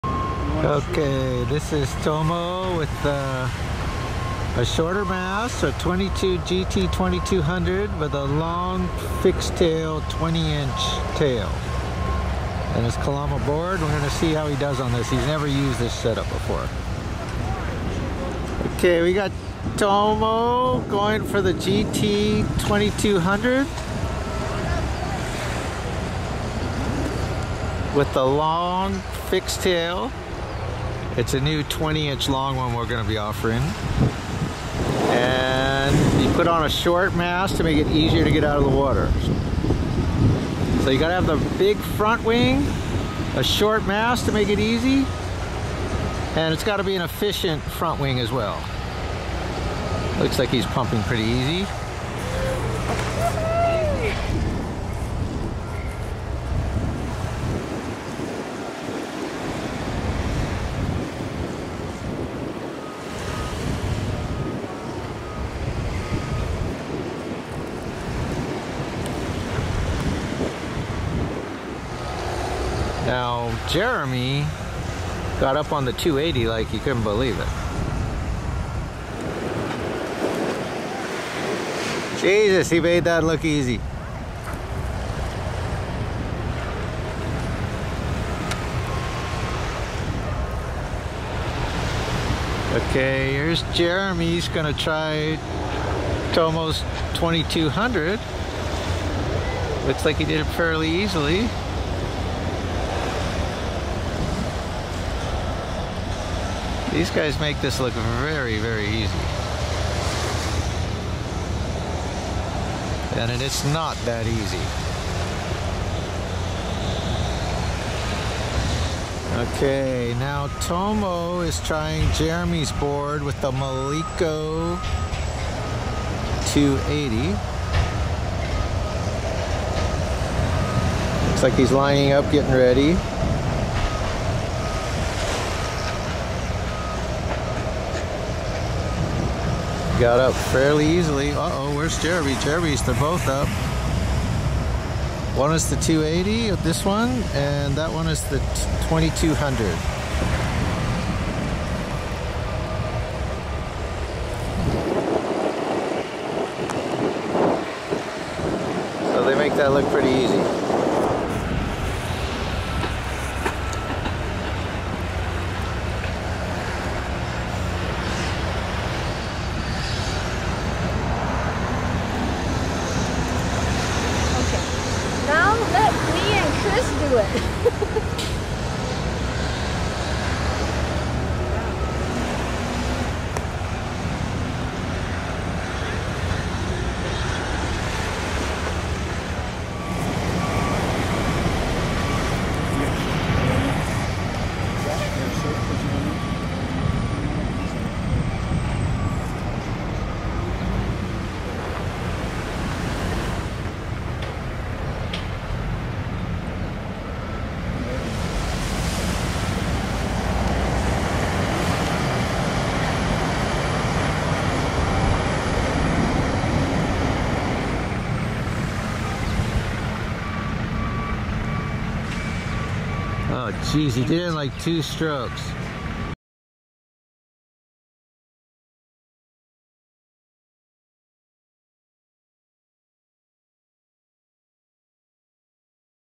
Flat Water foiling techniques
Jeremy Riggs and Tomo Murabayashi demonstrate how to get up in Flat water with just a paddle..